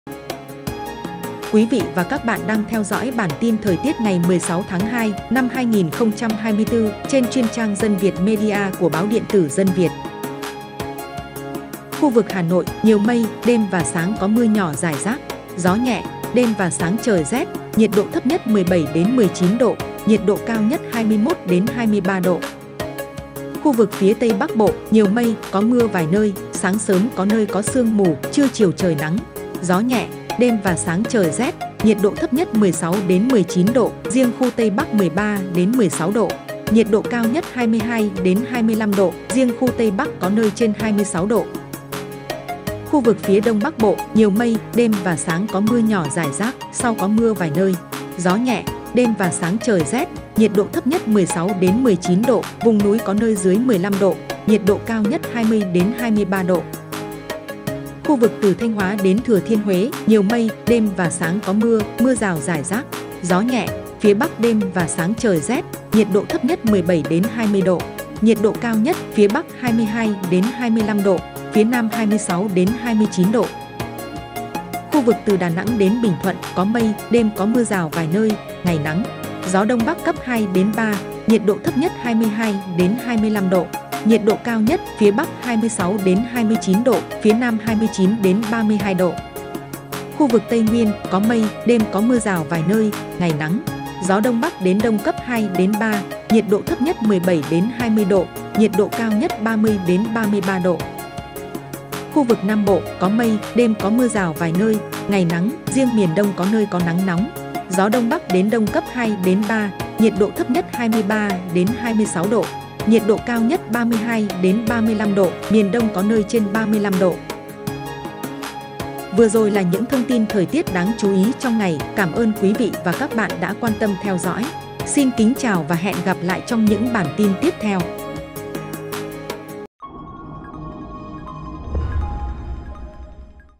Dự báo thời tiết hôm nay 16/2: Trung Bộ có mưa rào, Nam Bộ ngày nắng l Báo Dân Việt
Báo điện tử Dân Việt | Theo Trung tâm Dự báo Khí tượng Thủy văn Quốc gia: Bắc Bộ, Bắc Trung Bộ đêm và sáng trời rét; Đông Bắc Bộ mưa nhỏ rải rác; Tây Bắc Bộ sáng sớm sương mù, trưa, chiều hửng nắng. Bắc Trung Bộ mưa rào rải rác. Nam Trung Bộ, Tây Nguyên, Nam Bộ ngày nắng; Đông Nam Bộ có nắng nóng.